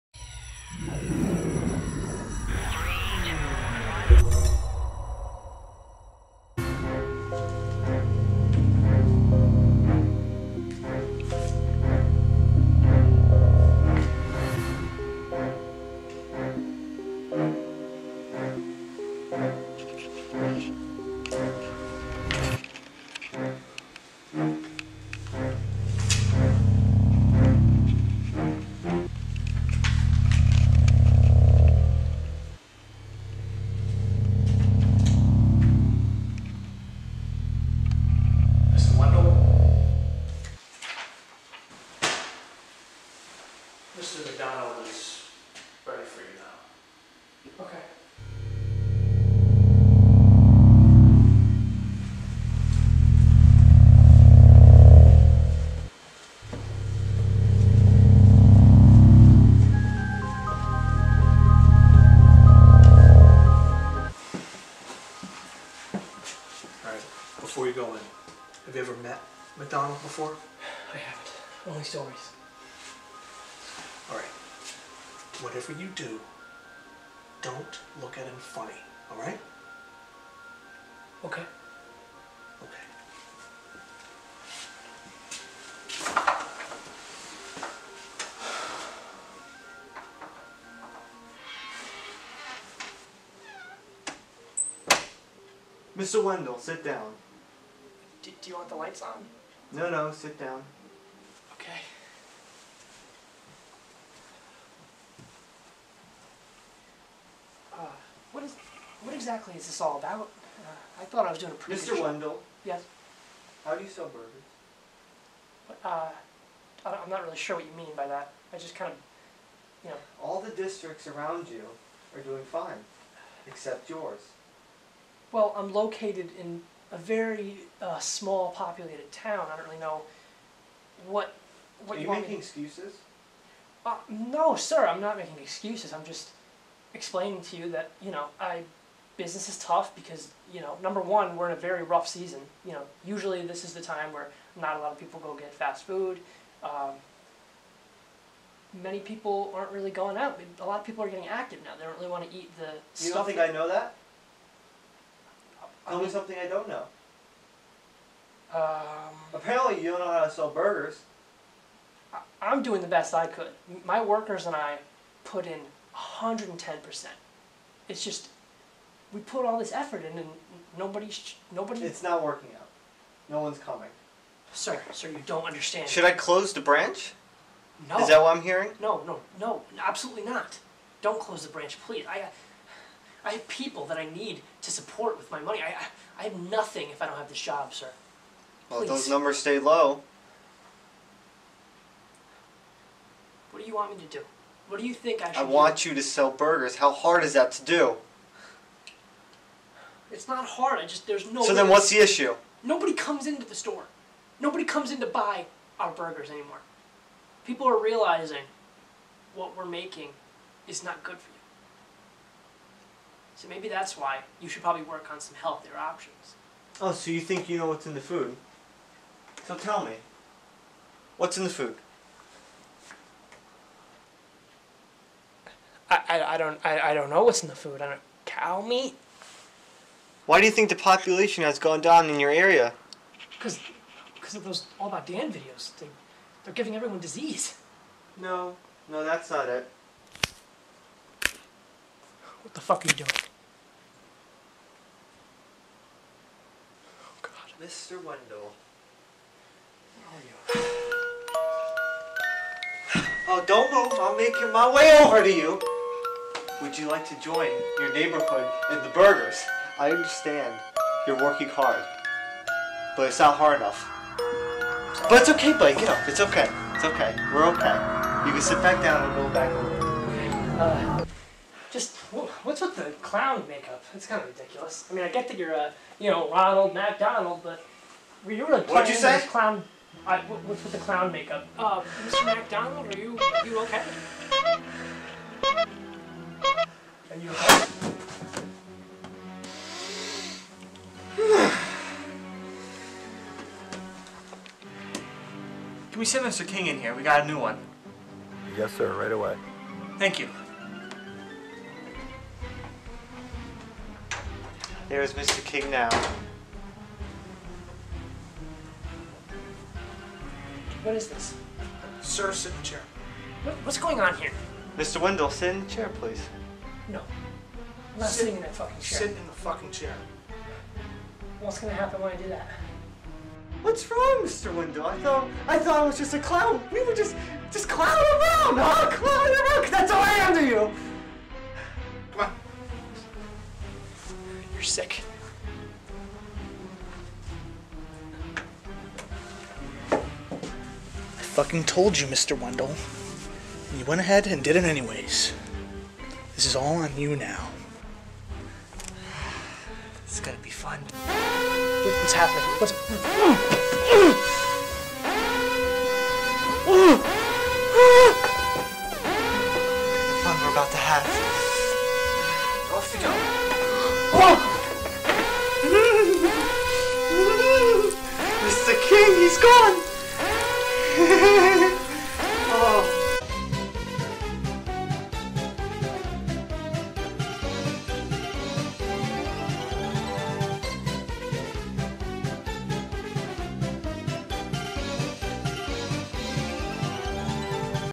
MacDonald's
MacDonald's is a spoof on Ronald McDonald. Mr. Wendell gets called in for a performance review, as things progress, secrets about the restaurant chain are revealed.

Have a Happy Thanksgiving!!